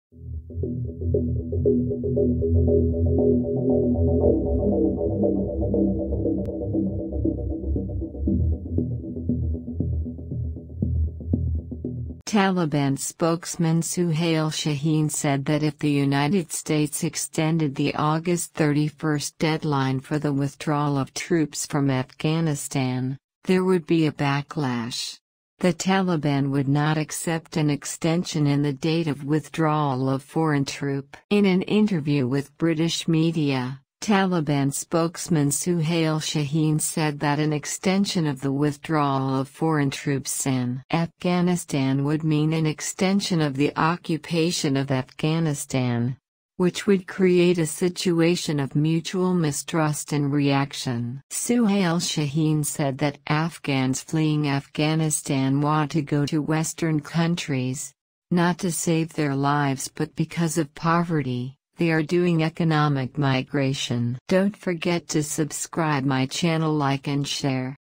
suhali shaeeni | US extends the withdrawal deadline | 60 NEWS
Sohail Shaheen has said that Afghan migrants are leaving for western countries due to poverty Migrants Taliban#

 My channel Name | 60 News